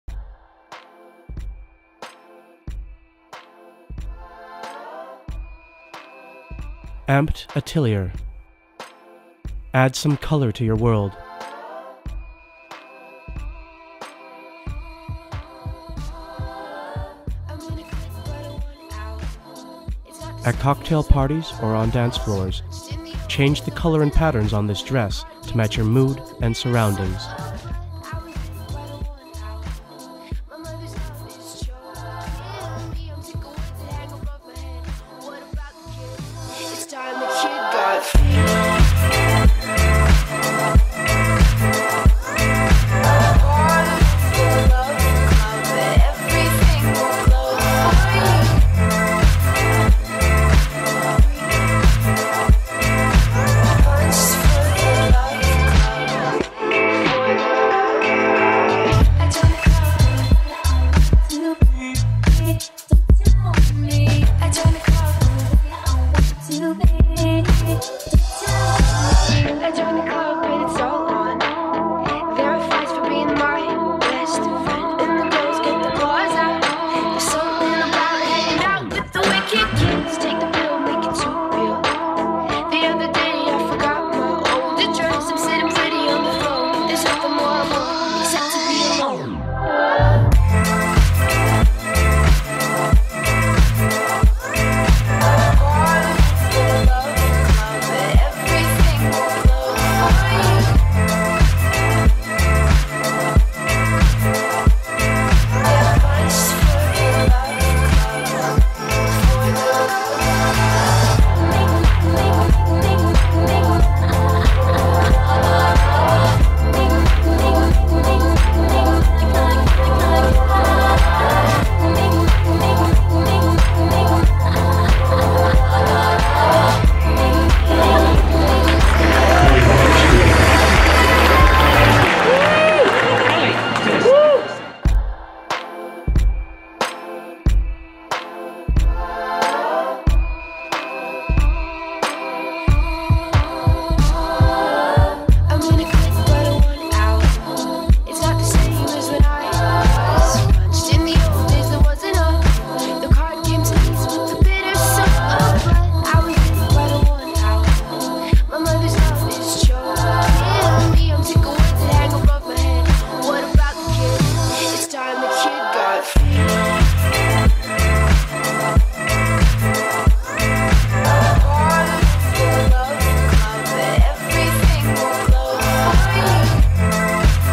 Amped Atelier: Mimic - MakeFashion 2016
MakeFashion 2016 April 2 2016

Amped Atelier"Mimic"

Sahrye Cohen and Hal Rodriguez are pleased to submit a thrilling project for the 2016 MakeFashion Gala!

Mimic is a Chameleon dress, changing colors in reaction to the environment. The dress uses sensors to measure object colors, changing the color of the dress through LED lights on 3D printed motifs. The wearer of the garment controls the color through the use of color swatches.

Designers: Sahrye Cohen and Hal Rodriguez